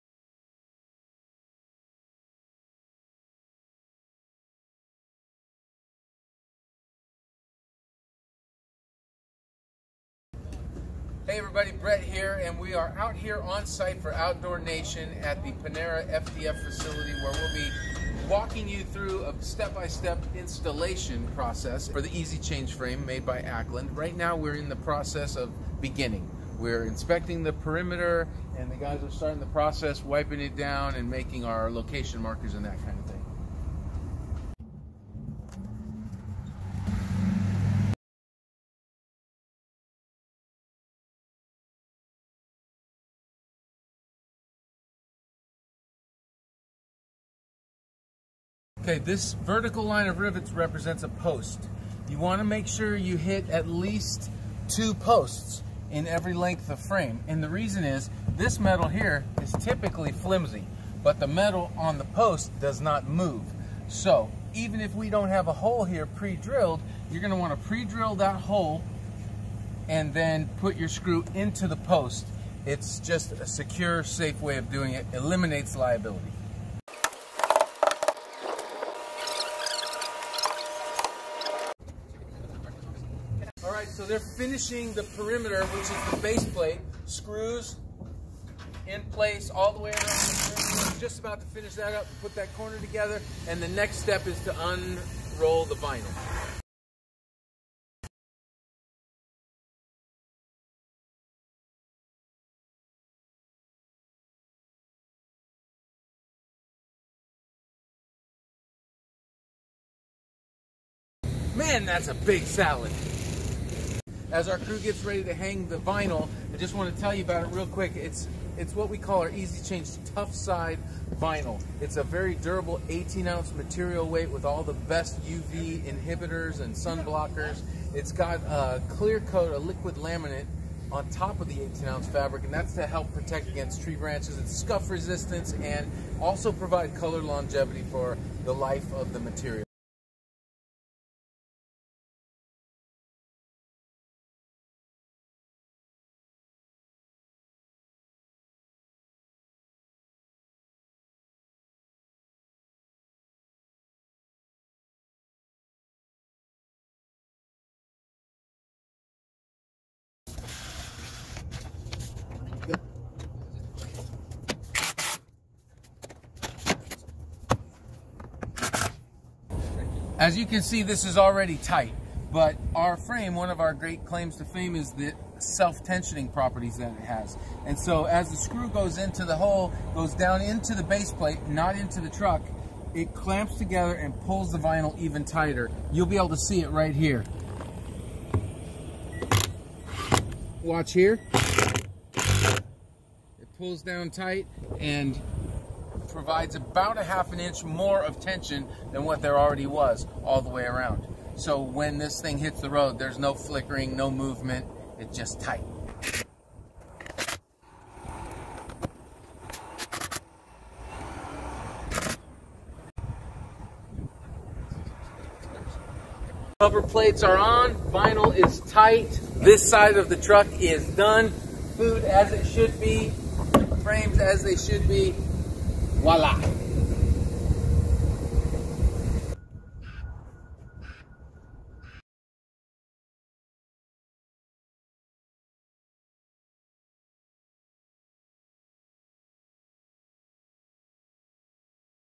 On-Site Truckside Installation (Abbreviated) - Panera, Outdoor Nation, Ackland
Quick Install Summary: Start-to-finish! EZ-ChangeFleet Graphics manufactured by Ackland Frames, job and print produced by Outdoor Nation for Panera Bread Co.